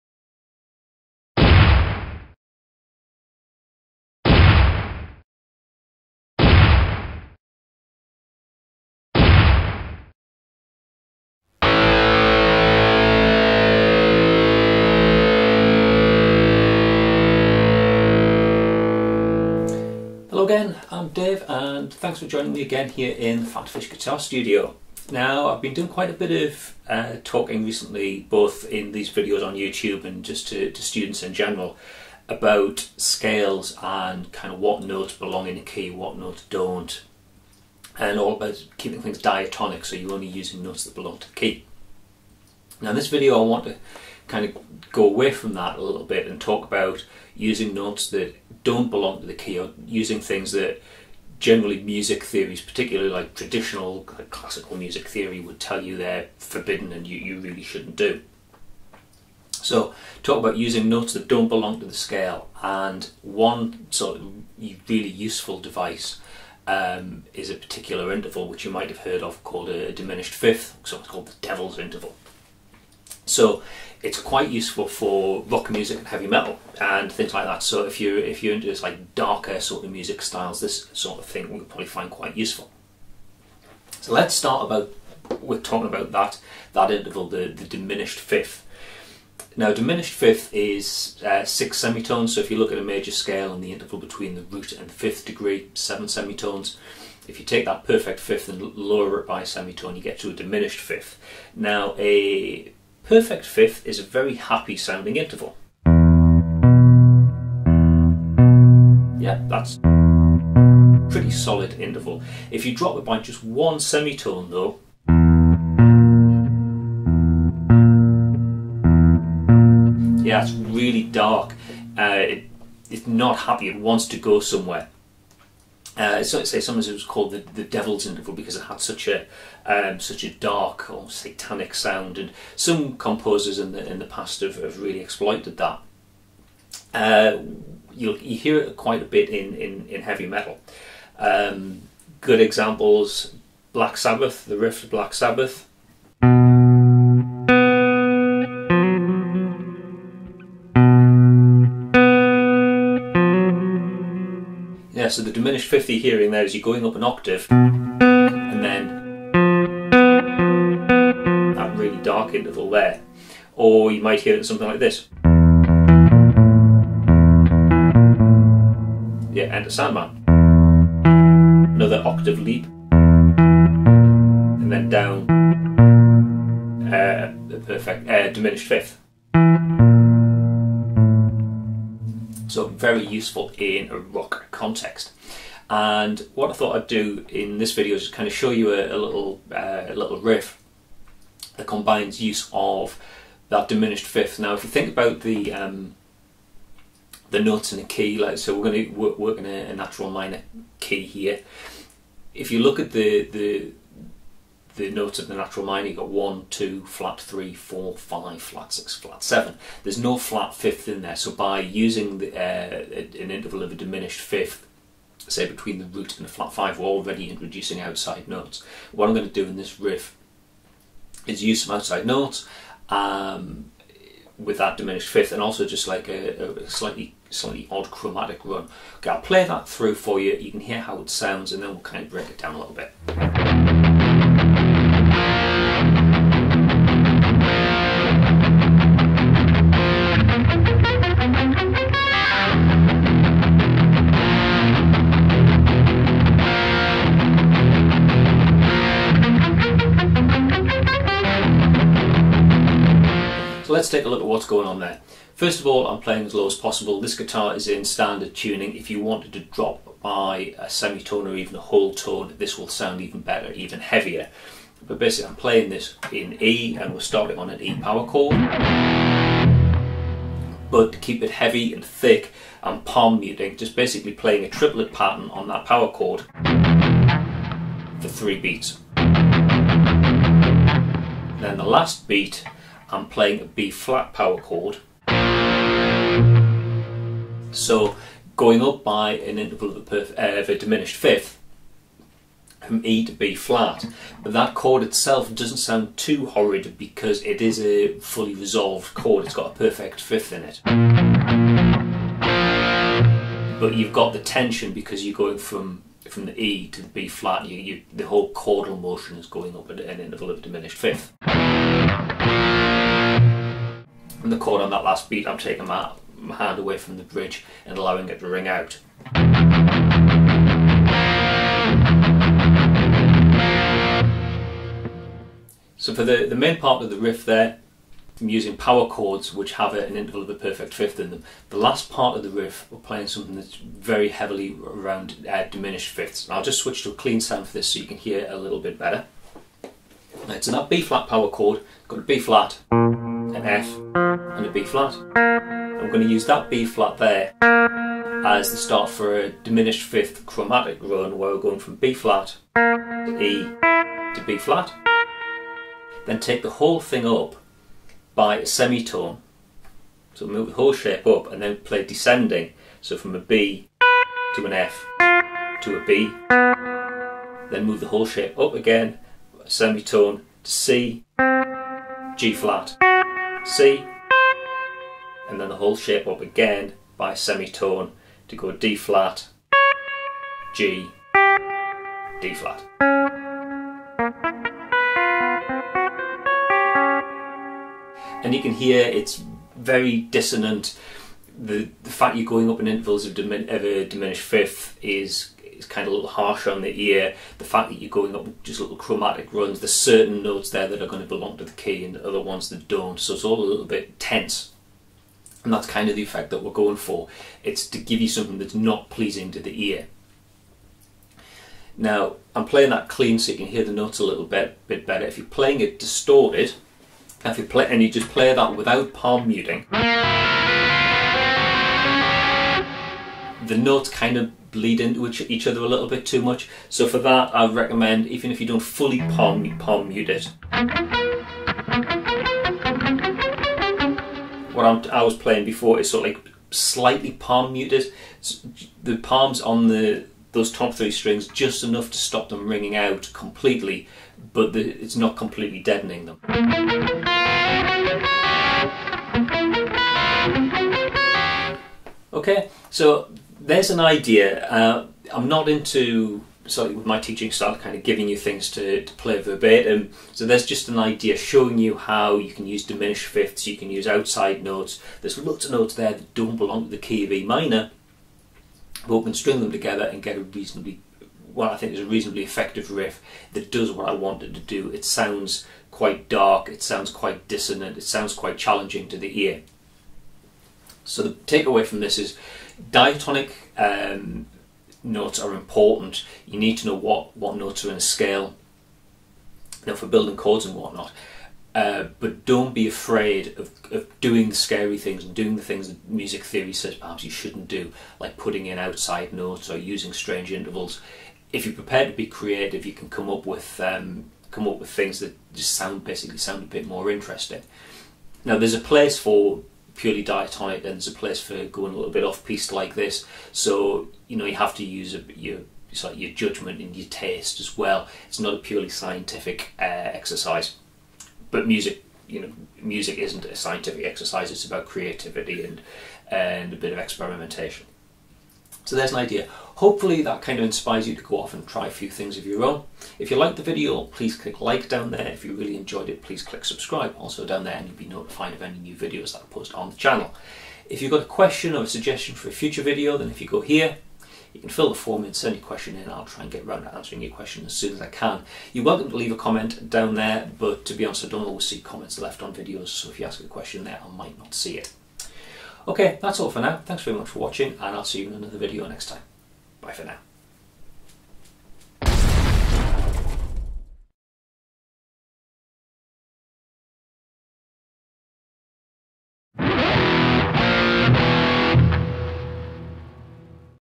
Odd Intervals And Outside Notes
When you study music theory you learn a lot about what notes are "allowed" to be used in a given situation.  However, you can open up whole new avenues of creativity by introducing outside notes and other "forbidden" musical devices.

Got a question?